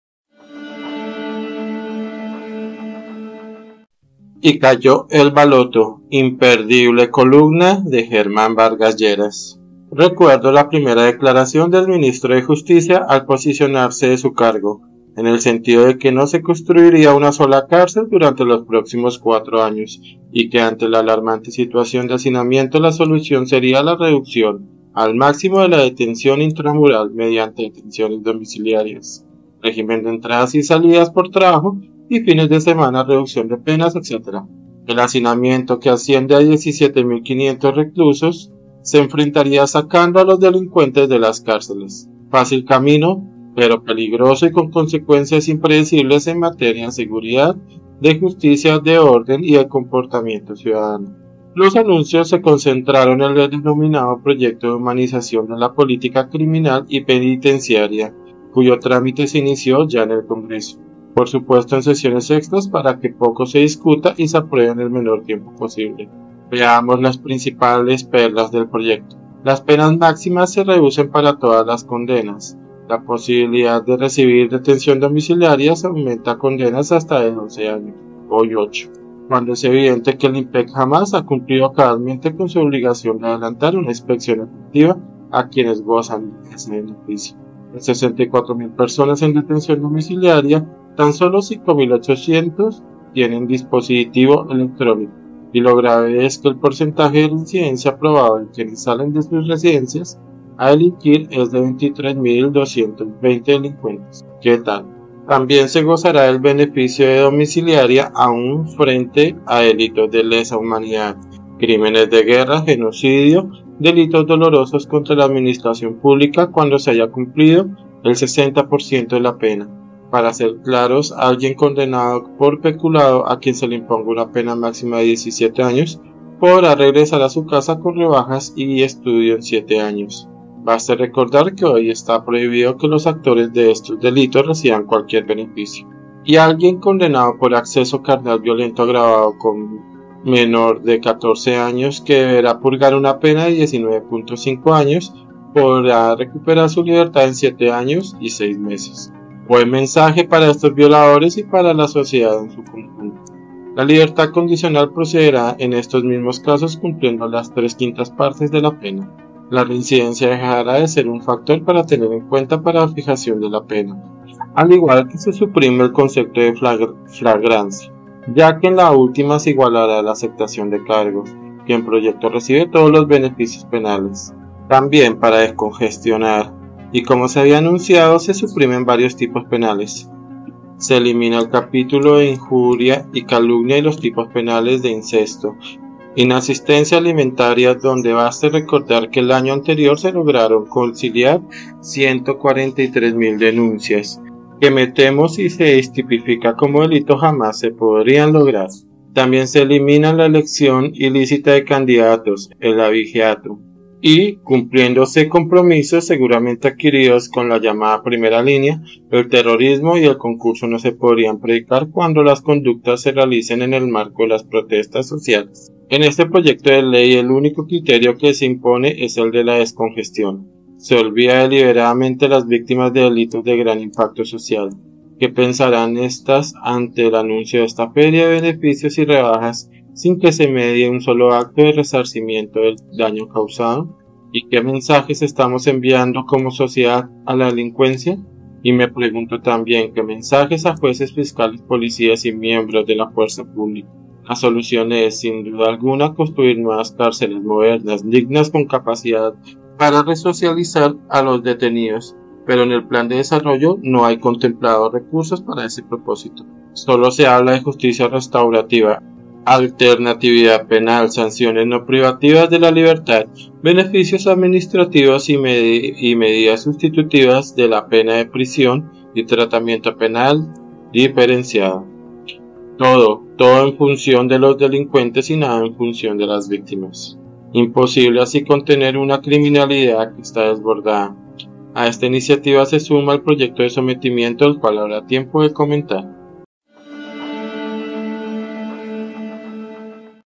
🔴 IMPERDIBLE COLUMNA VARGAS LLERAS: || "Y Cayó el Baloto"
Y cayó el baloto
La solución es construir nuevas cárceles, modernas, dignas, con capacidad para resocializar.
columna German Vargas Lleras